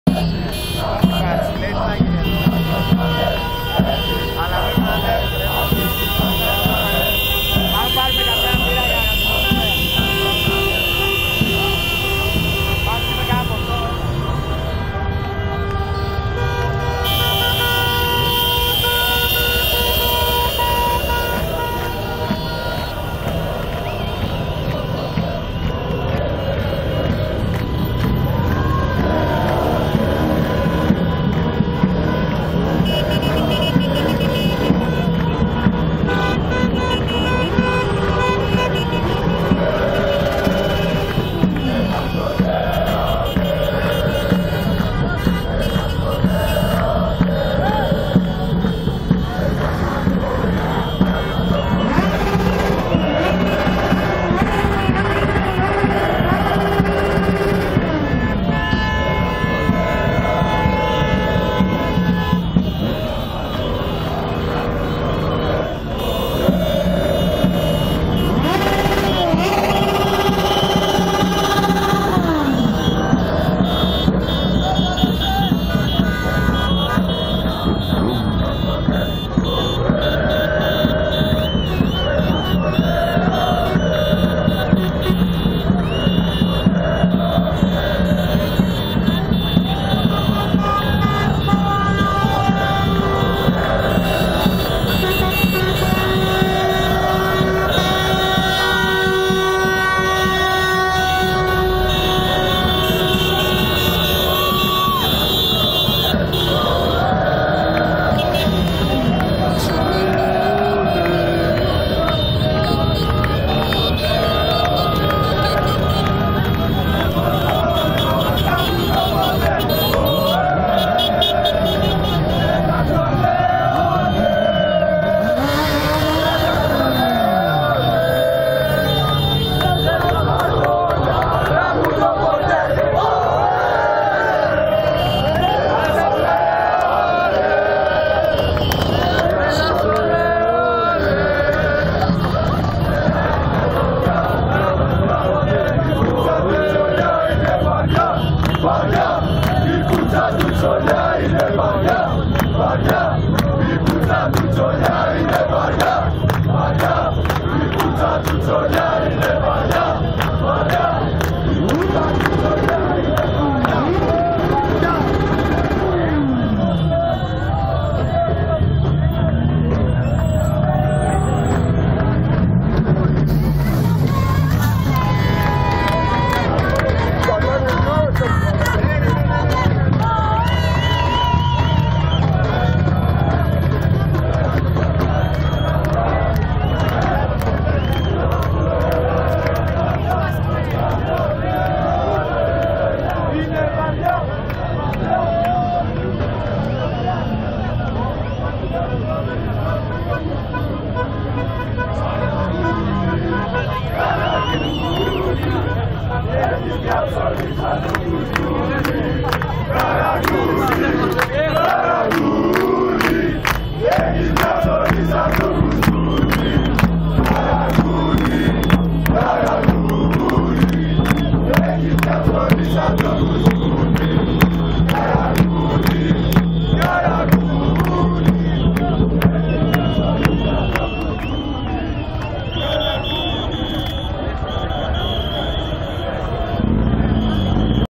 ΠΑΝΗΓΥΡΙΣΜΟΥΣ ΣΤΗΝ ΑΘΗΝΑ META THN ΠΡΟΚΡΙΣΗ ΤΗΣ ΕΘΝΙΚΗΣ ΕΛΛΑΔΟΣ ΣΤΙΣ 16 ΚΑΛΥΤΕΡΕΣ ΟΜΑΔΕΣ ΤΟΥ ΠΛΑΝΗΤΗ
GREEK FAN CELEBRATIONS IN ATHENS AFTER THE QUALIFICATION TO THE 16 BEAST TEAMS OF THE PLANET FIFA WORLD CUP BRAZIL 2014 🏆⚽🏃